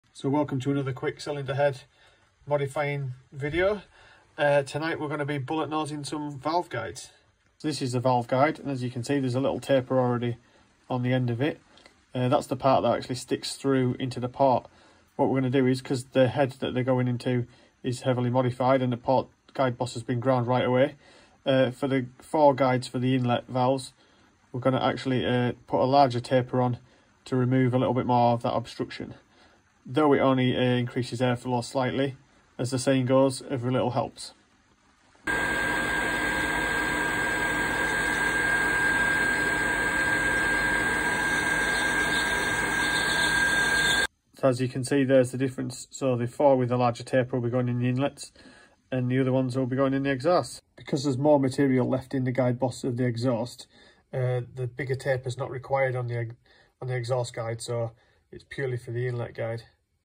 Bullet nosed valve guides how to Improve airflow on your classic mini cylinder head.
How to bullet nose valve guides to improve airflow through your classic Mini cylinder head to help increase power output and performance.